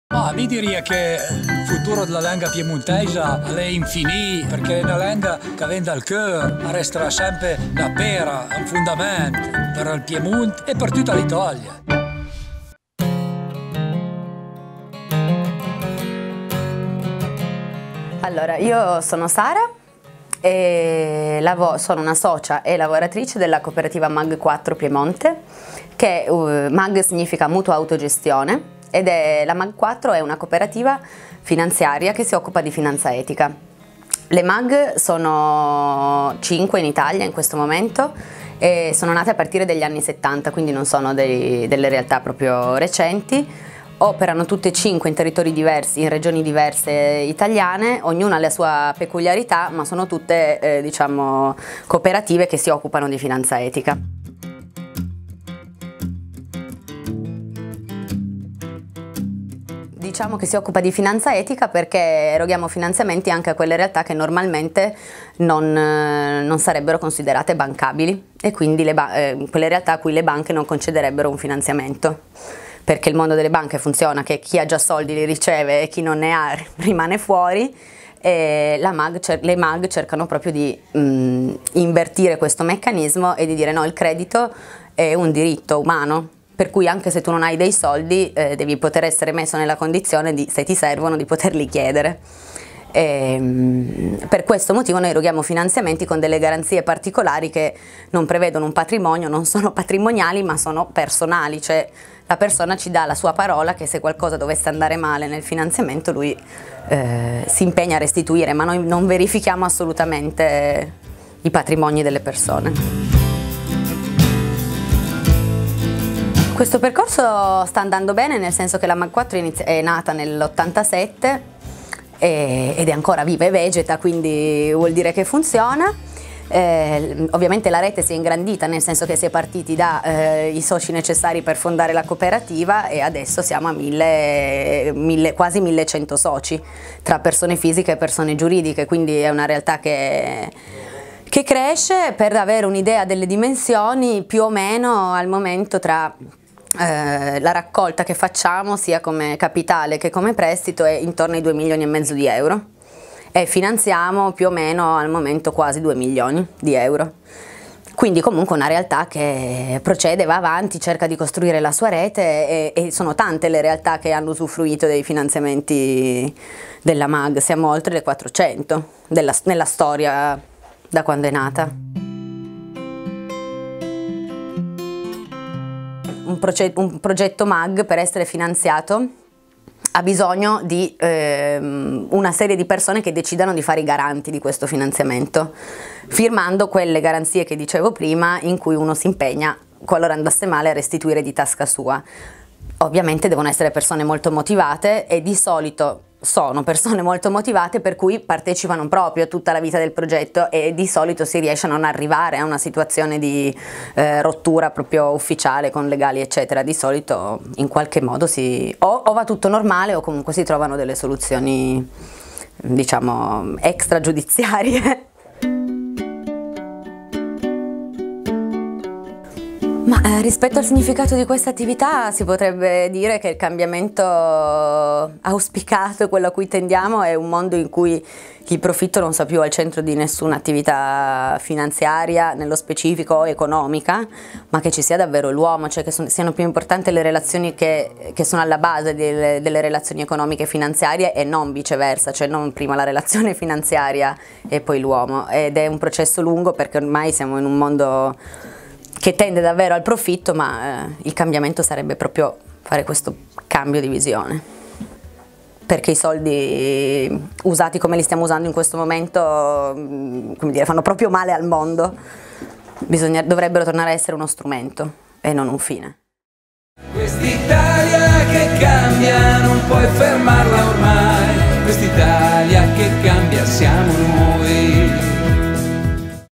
Mag4, quando l’economia mette al centro l’uomo e non il profitto
Dagli anni settanta la cooperativa cerca di invertire questo meccanismo affermando che il credito è un diritto umano. La Mag4 finanzia attività che portano avanti un modello diverso di economia, un economia che metta al centro l’uomo e non il profitto.

Intervista e realizzazione video: Roberto Vietti